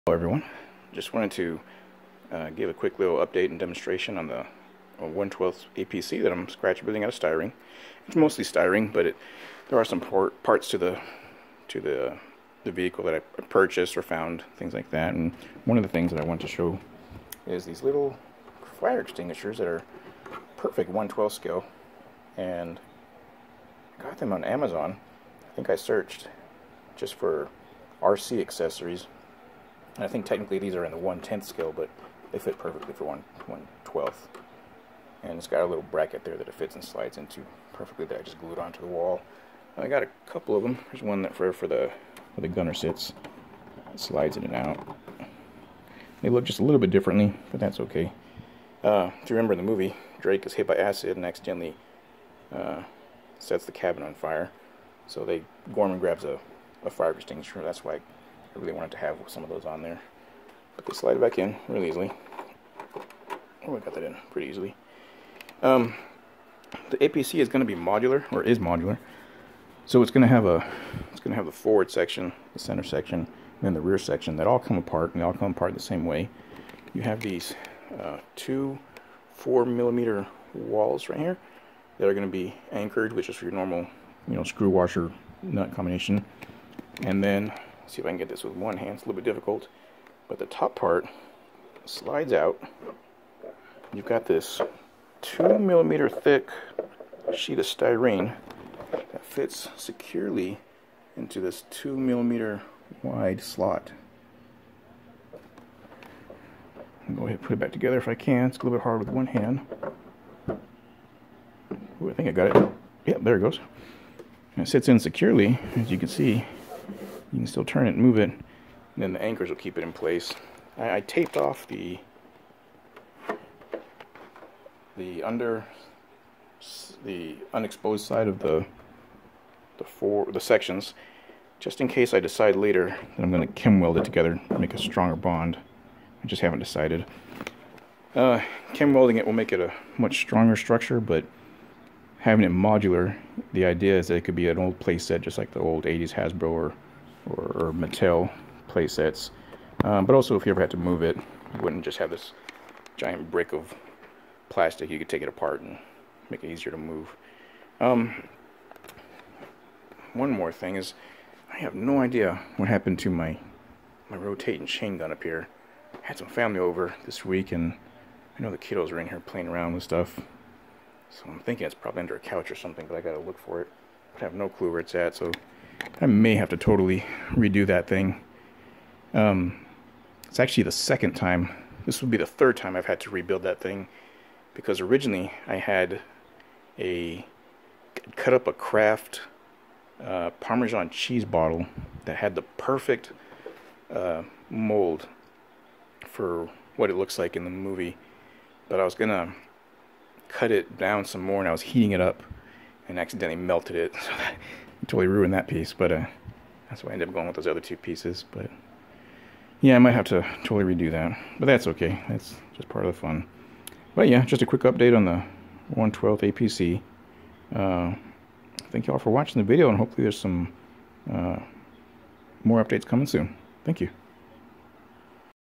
Modular Aliens APC playset demonstration.
Demonstrating how my scratch build APC pieces together and can be taken apart. Also some cheap accessories I found online. I dont know why this recorded in such poor/ low video quality. Might have to reupload.